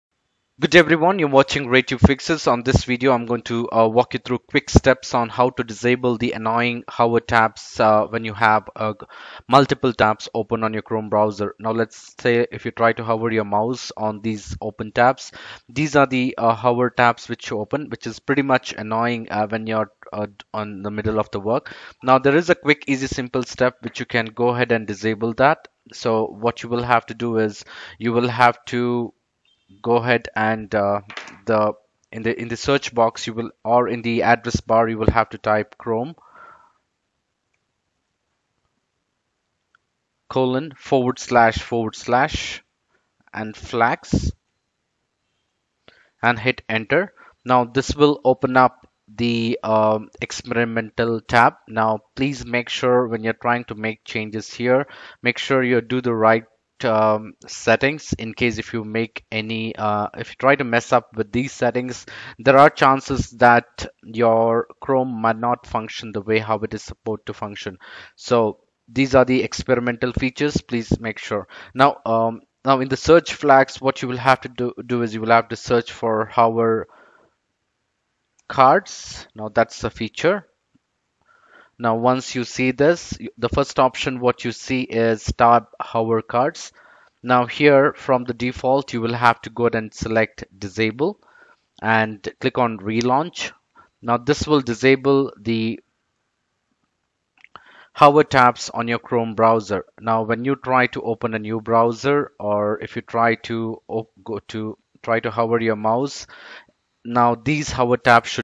How to Disable Tab Hover Cards on Google Chrome
How to disable Tab Hover Cards feature in Chrome
To disable Tab Hover Cards in Chrome, use these steps:

Type the following path in the address bar and press Enter:

chrome://flags/tab-hover-cards

Use the drop-down menu on the right and select the Disabled option.

chrome://flags/tab-hover-card-images

Click the Relaunch now button.

Once you complete the steps, when you hover over the other tabs should no longer display a card preview.

If you want to re-enable the Tab Hover Cards, go to the same settings and click on enable :).